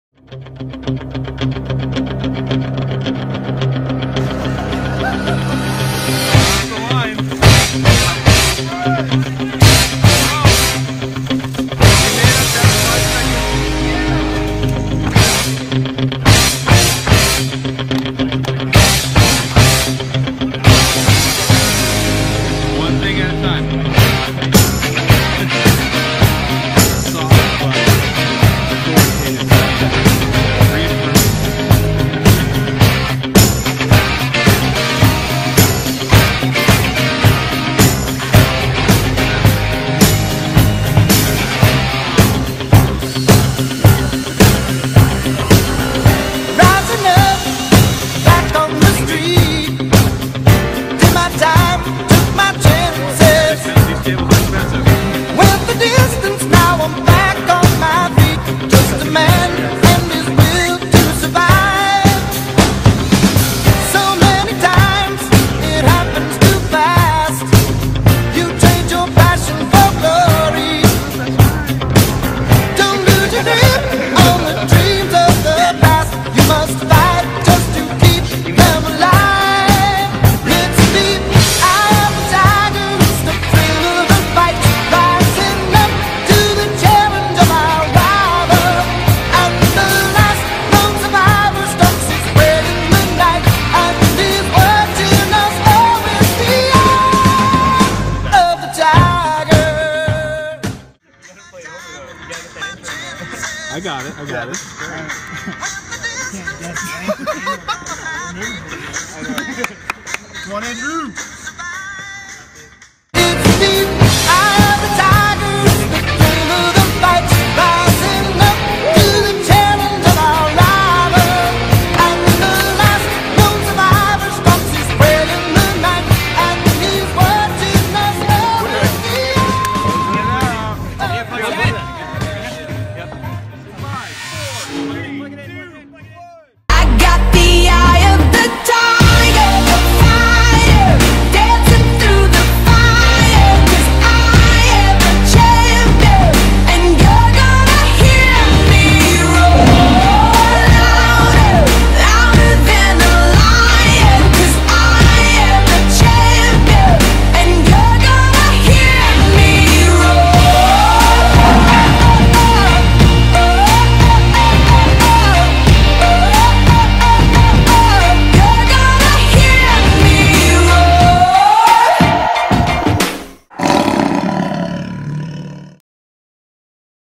Cali Display Jam Out: Display Olympics Mystery Build Relay
OC/South Bay, LA, and Valley district DA's got together in December for a day of inspiration, discussion, donuts, and at the end of the day, DISPLAY OLYMPICS! Check out the video for a recap of the Mystery Build Relay that was the main event.